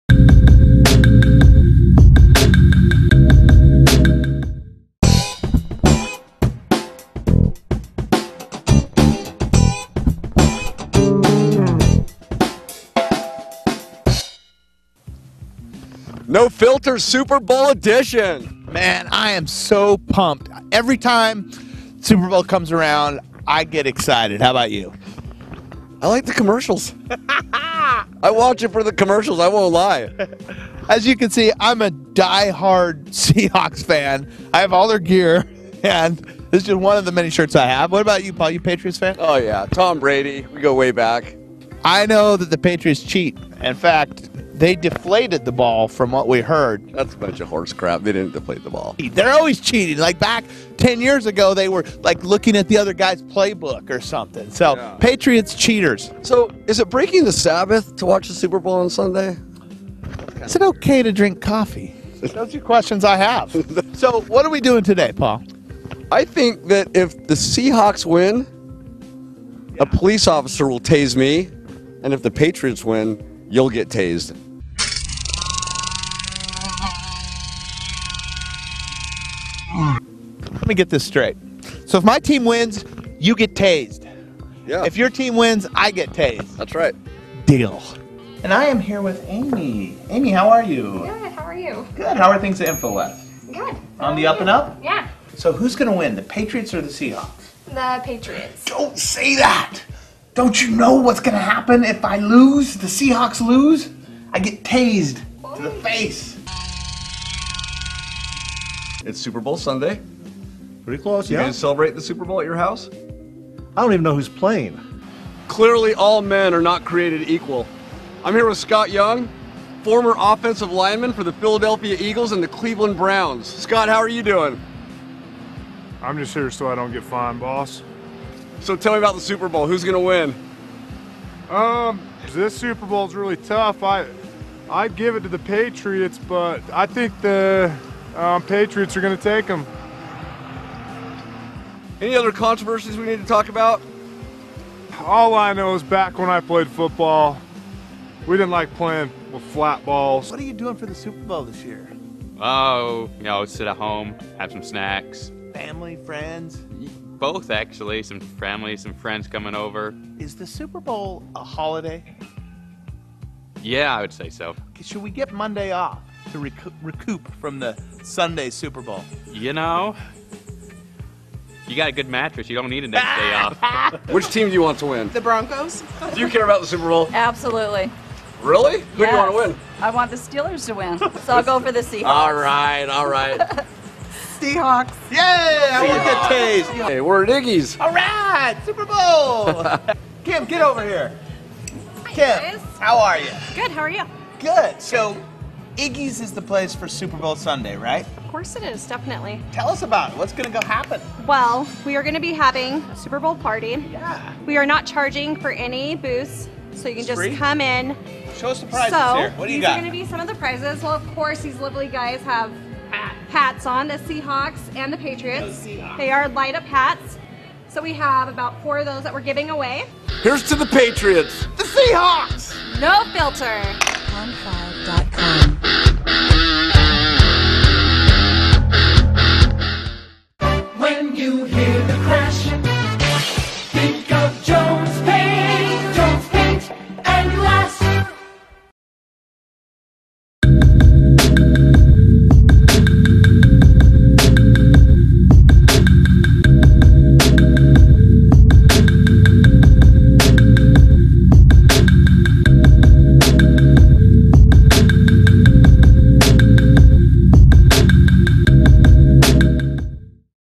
No Filter: Patriots or Seahawks? Super Bowl XLIX
For some, it’s about the game. For others, it’s about the commercials or fried chicken or wings.  It’s America’s NFL Super Bowl, and it’s time to pick your team. Co-hosts Paul Ford and Grady Sinclair have made their picks and lay it all on the line with an unusual wager in which the loser will suffer the ultimate penalty. Don your jerseys and show your team spirit with Paul and Grady in this Episode 22 of the”No Filter Show.”